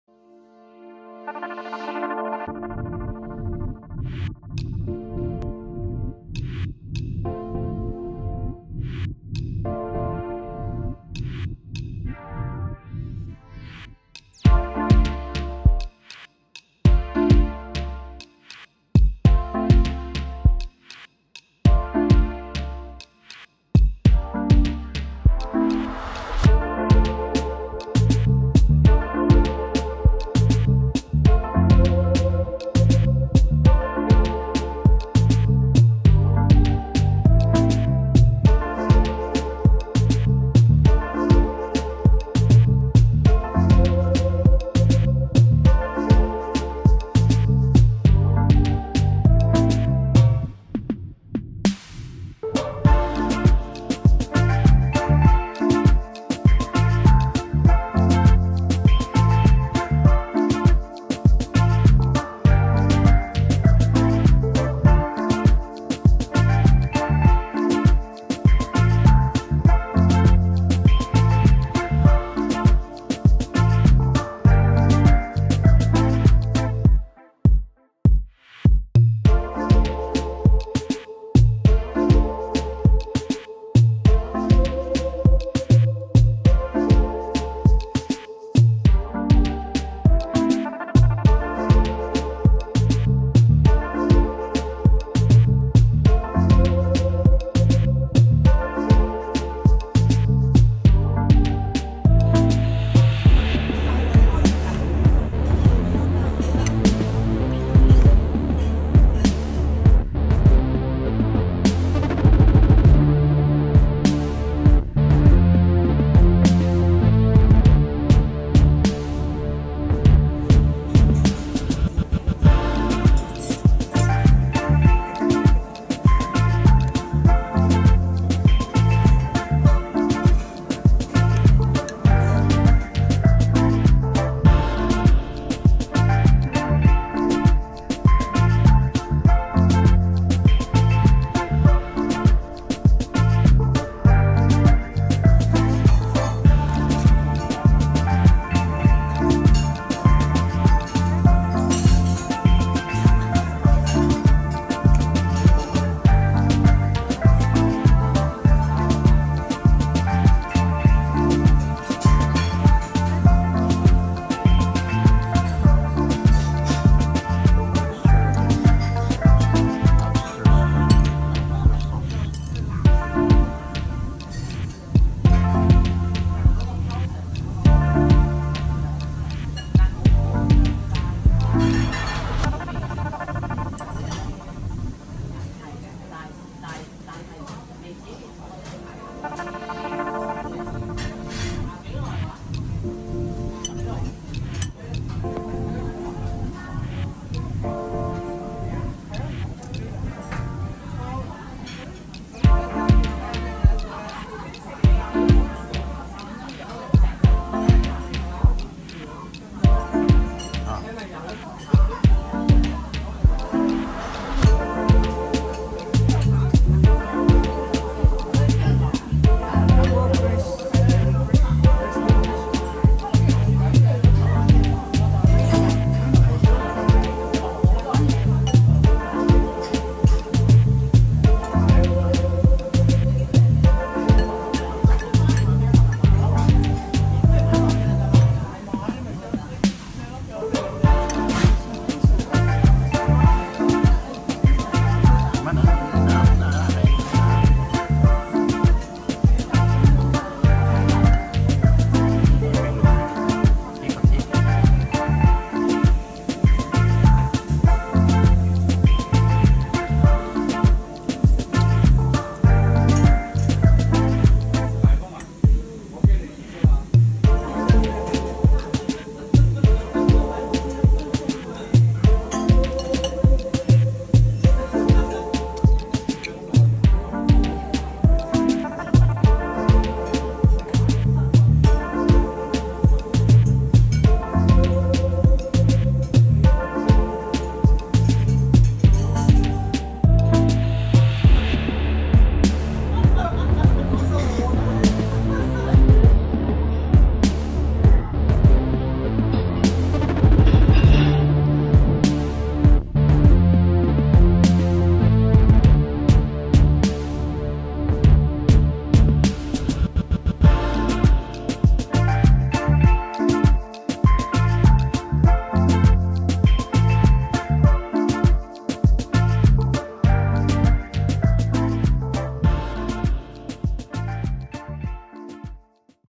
Sumptuous Set Lunch At ToTt Terrace-Pinoy Outdoor Oldies Food Vlog #22
Halloween is a time to celebrate the day before All Saint's Day where the young and old alike wear costume on the street or on private parties. In celebration we decided to have our lunch at ToTt Terrace in Causeway Bay, Hong Kong! Join us as we enjoyed our sumptuous set lunch with warm hospitality from the Proprietor himself!  Clean cozy and very welcoming service awaits you! Please watch the entire video and thank you very much!

Video editor and Audio:
From iMovie apps and Studio.Youtube.com